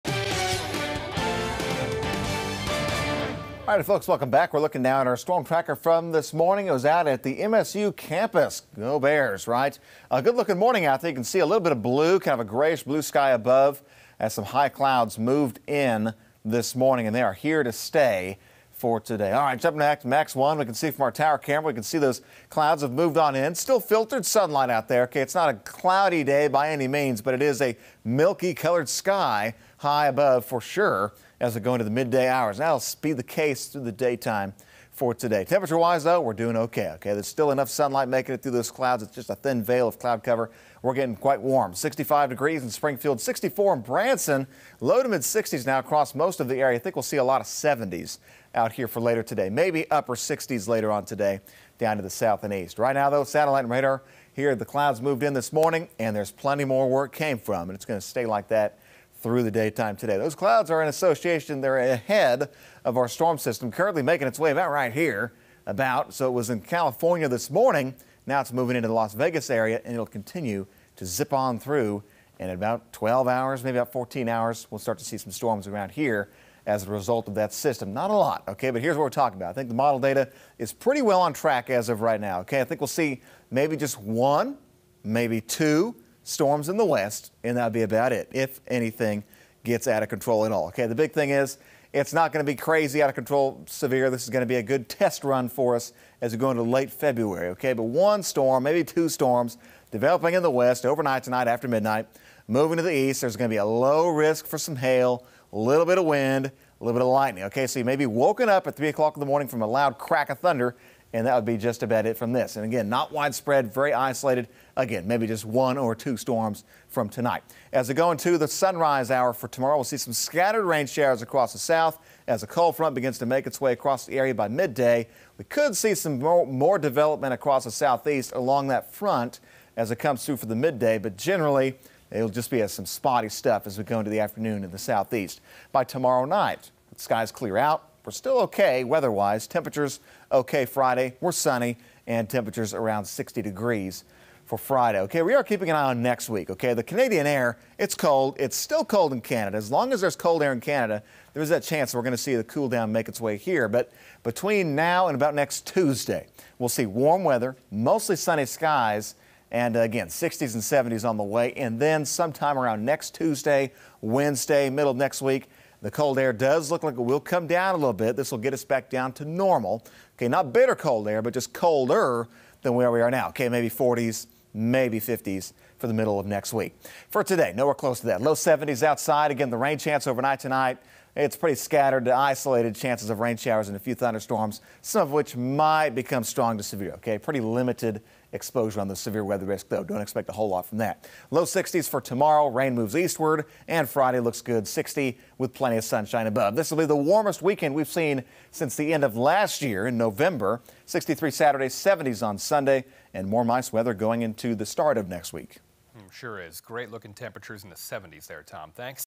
Spring-feel sticks around despite upcoming cold front
Wednesday, February 21 | Tom's Midday Forecast